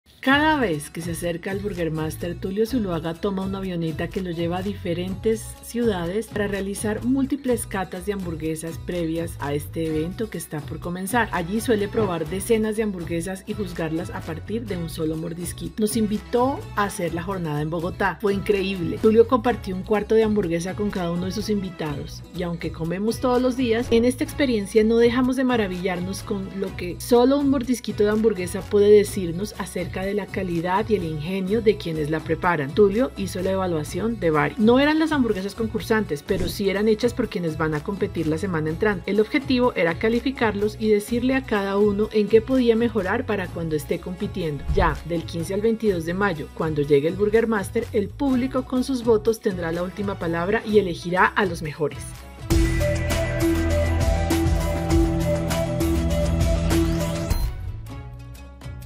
Así evalúa Tulio Zuloaga las recetas del Burger Máster El Tiempo
Poco antes del Burger Máster, Tulio comparte las lecciones de la experiencia de este evento.